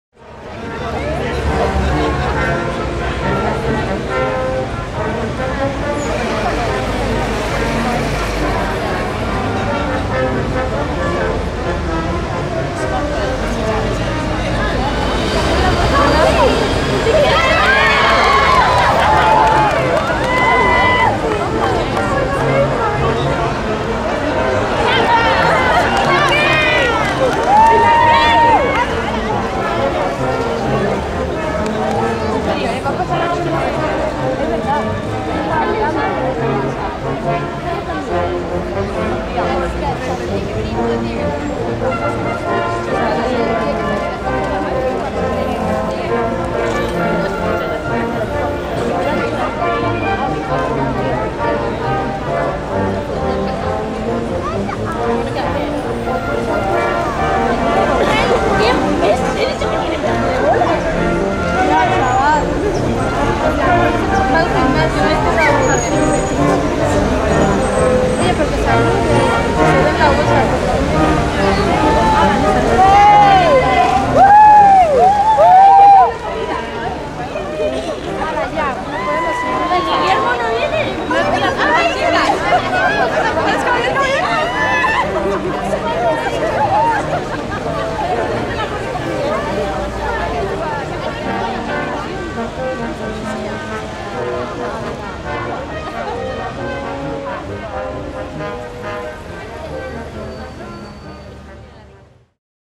Regent Hall Band at Fortnum & Masons with the Queen, Camila & Catherine
A group of players from Regent Hall Band of The Salvation Army in London played for the Queen as she visited Fortnum & Mason's store on Piccadilly in London with Camila and Catherine.